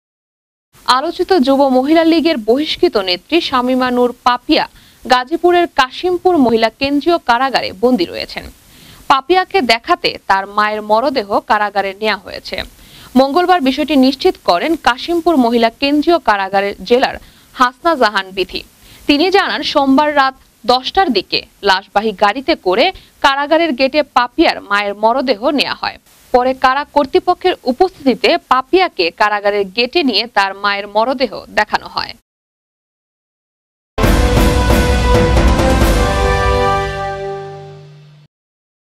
কারাগারের গেট থেকেই মায়ের মরদেহ দেখলেন পাপিয়া
আলোচিত যুব মহিলা লীগের বহিষ্কৃত নেত্রী শামীমা নূর পাপিয়া গাজীপুরের কাশিমপুর মহিলা কেন্দ্রীয় কারাগারে বন্দি রয়েছেন। পাপিয়াকে দেখাতে তার মায়ের মরদেহ কারাগারে নেওয়া হয়েছে। মঙ্গলবার  বিষয়টি নিশ্চিত করেন কাশিমপুর মহিলা কেন্দ্রীয় কারাগারের জেলার হাসনা জাহান বিথী। তিনি জানান, সোমবার রাত ১০টার দিকে লাশবাহী গাড়িতে করে কারাগারের গেটে পাপিয়ার মায়ের মরদেহ নেওয়া হয়। পরে কারা কর্তৃপক্ষের উপস্থিতিতে পাপিয়াকে কারাগারের গেটে নিয়ে তার মায়ের মরদেহ দেখানো হয়।